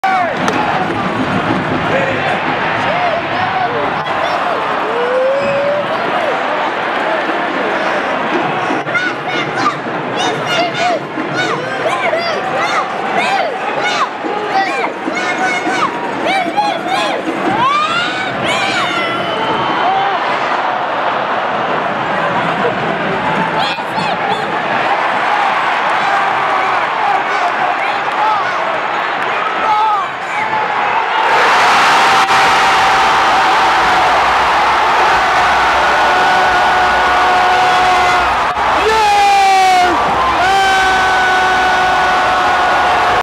game winner playoffs skc - madness
Game 2 of the playoff series against New England. Claudio Bieler puts Sporting KC ahead 4-3 on aggregate in the 114th minute!!! Advance Sporting KC to the next round of the playoffs vs Houston.